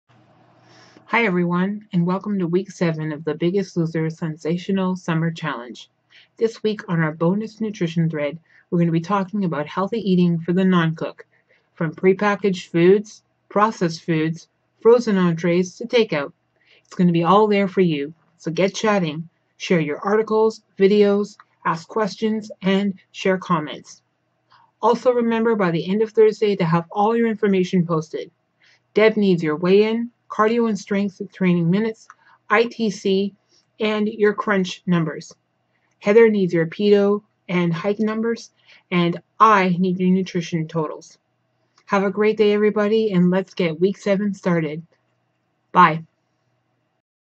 BLC Week 7 - Healthy Eating for the Non-Cook!
Here is our Week 7 Intro Video for the Biggest Loser Challenge Yellow Team!

Good Luck Yellows! Get those Numbers Posted!